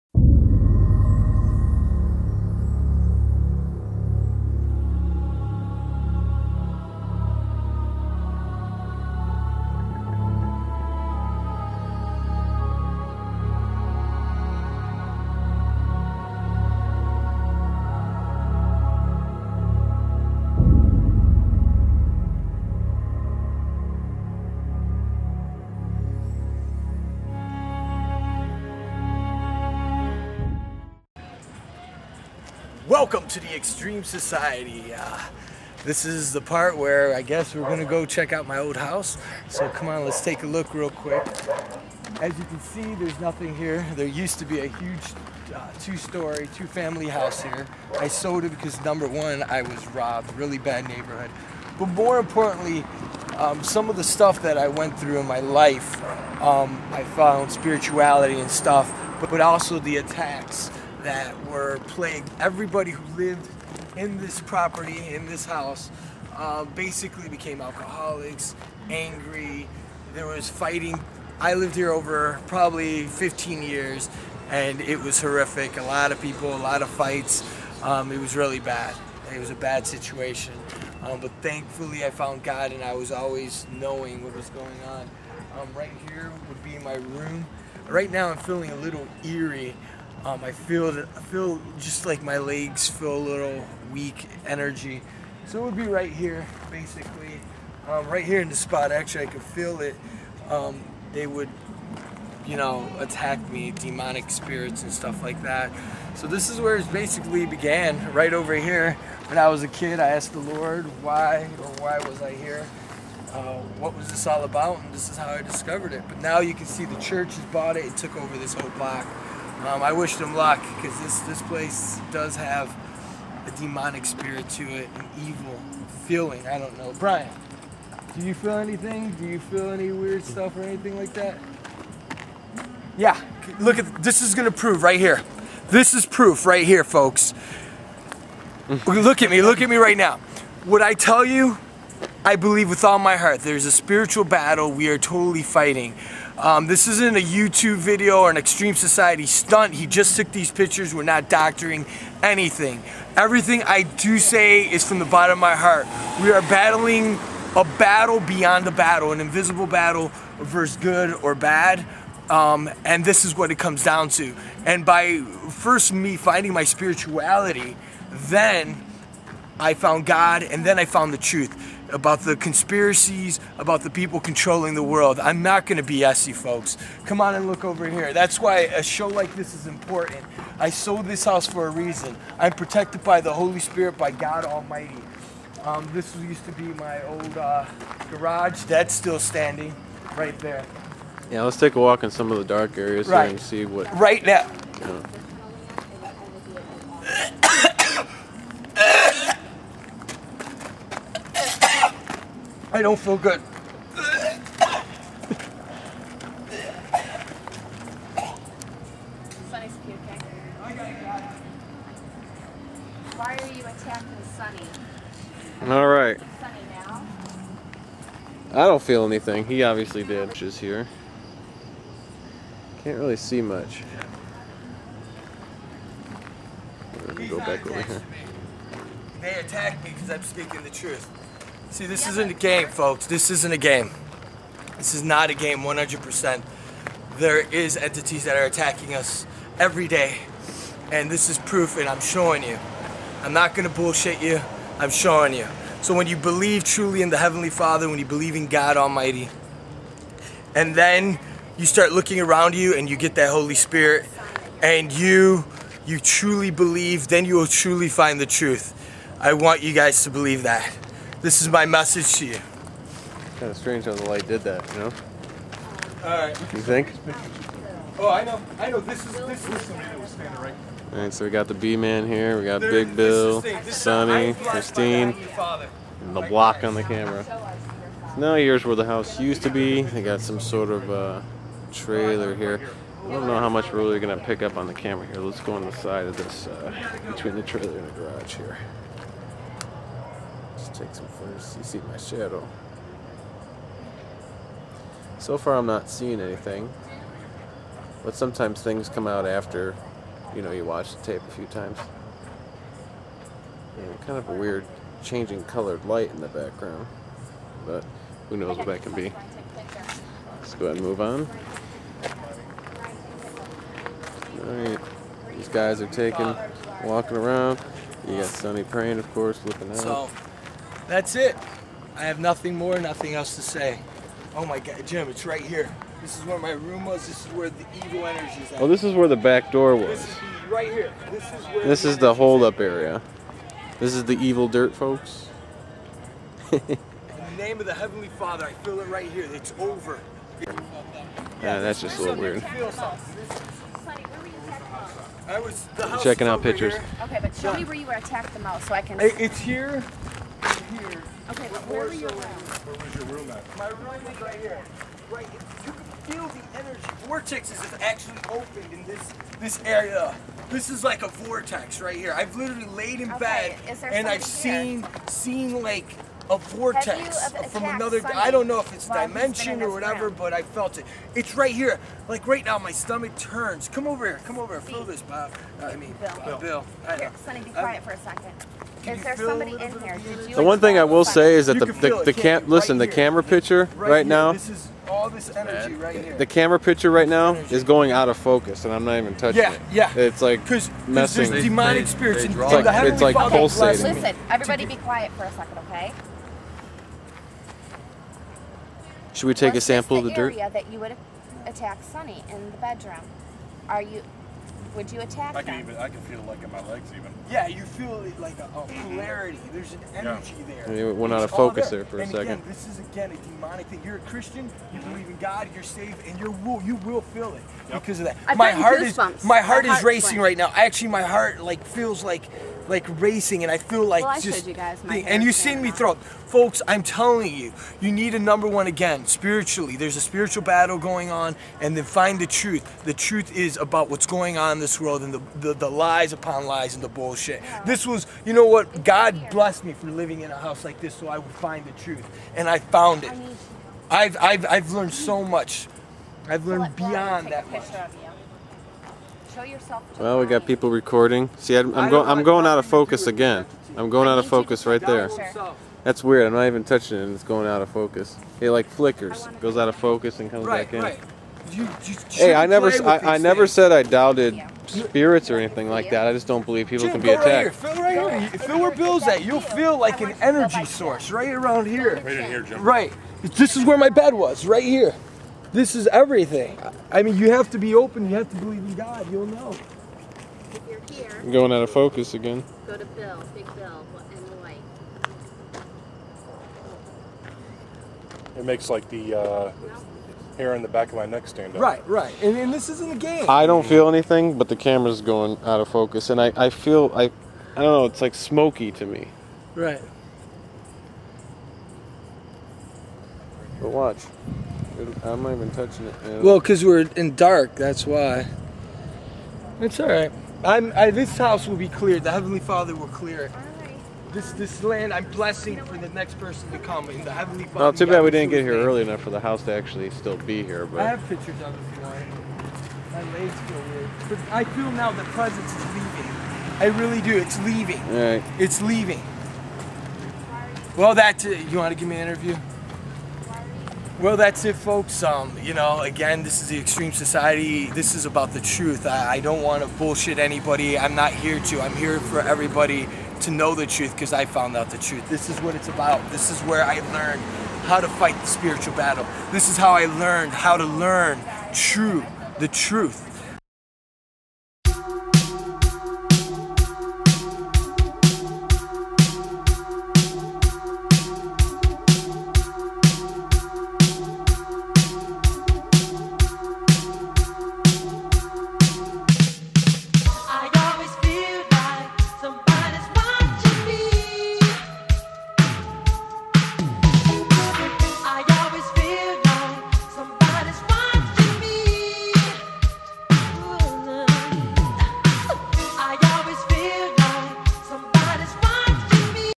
Haunted Grounds Video by The Extreme Society Show
We explore The co-host of the extreme societys old home where he claims he was bothered by demons...